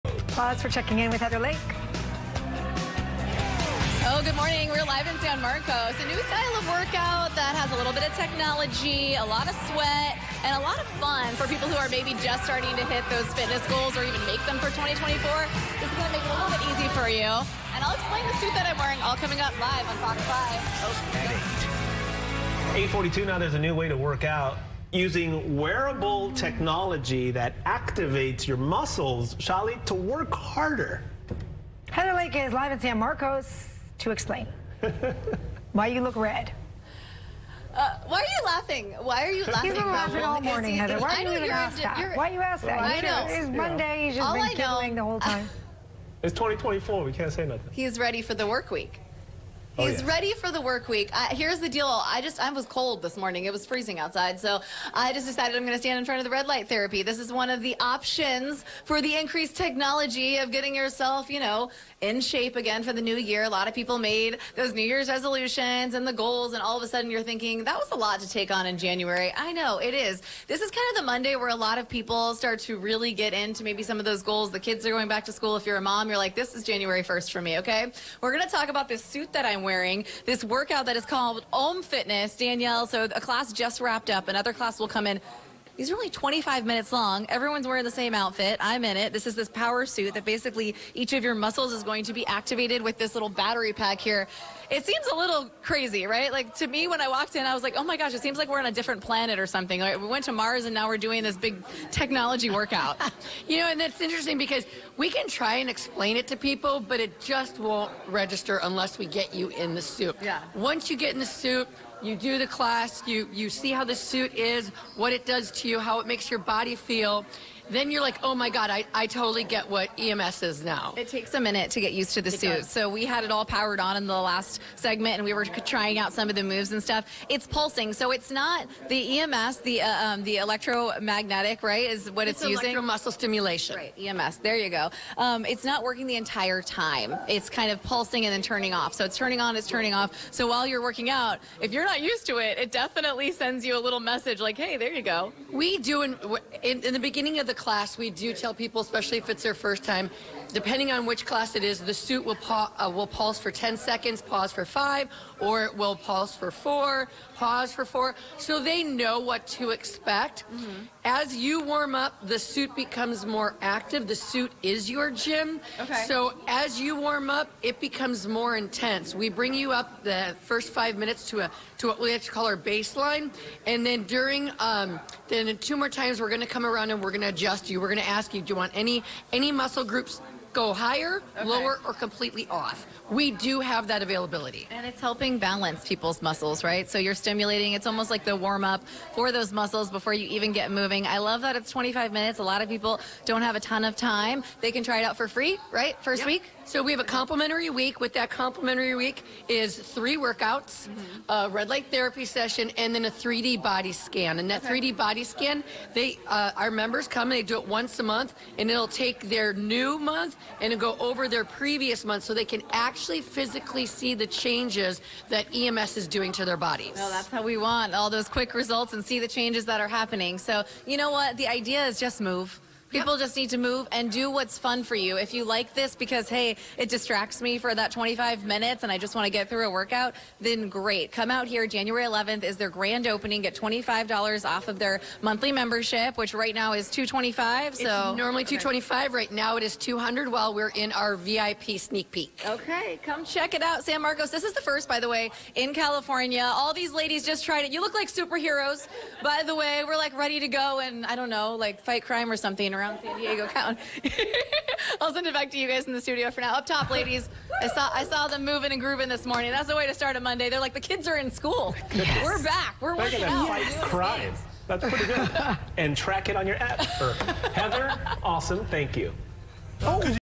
FOX Segment Previewing OHM Fitness Studio c3 Communications secured via Public Relations
In a recent TV segment on Fox 5 San Diego, reporter Heather Lake provided an exclusive preview of the innovative OHM Fitness Studio, a client of c3 Communications, Inc. Lake experienced firsthand the revolutionary EMS Technology workout, a cutting-edge fitness regimen that promises the benefits of a 2-hour workout in just 25 minutes. This unique approach to fitness is redefining the industry standards and attracting attention from fitness enthusiasts.

Lake also had the opportunity to interview Danielle Conklin, the dynamic owner of OHM Fitness Studio. Conklin shared insights into the workings of the studio and the benefits of the EMS Technology workout. The segment, secured by c3 Communications through strategic public relations, effectively highlighted the unique selling points of OHM Fitness Studio, reinforcing the importance of public relations in shaping public perception and awareness.